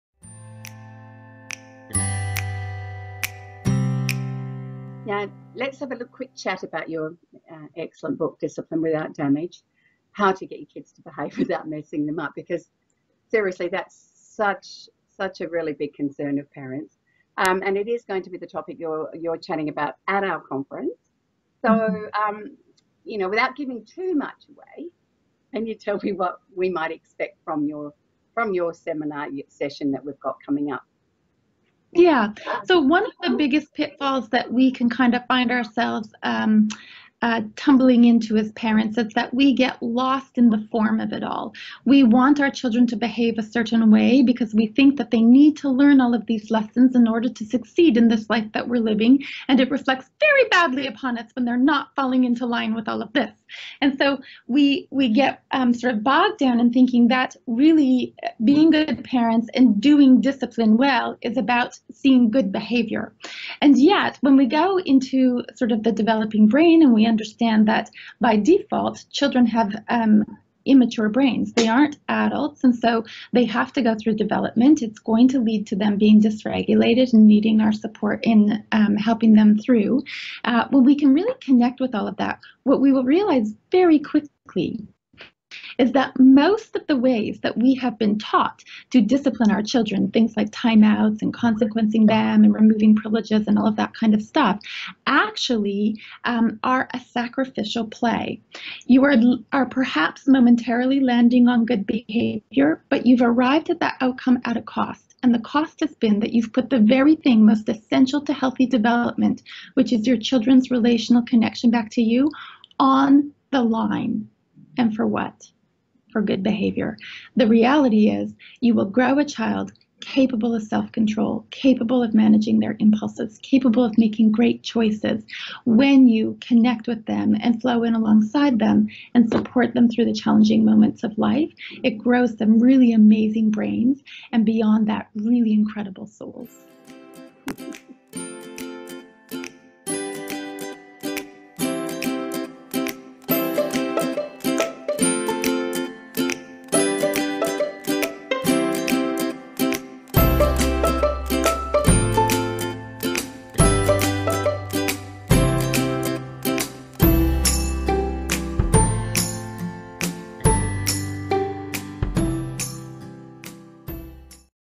How to discipline kids without messing them up - Maggie Dent & Dr Vanessa LaPointe #3
This is a clip from an interview between one of Australia's favourite parenting authors Maggie Dent and author of "Discipline Without Damage", Dr Vanessa LaPointe.

Maggie is bringing Vanessa to Australia in March 2018 for her RAISING CHILDREN WHO SHINE conferences, exploring toddlers to 10-year-olds for parents and people who work with children.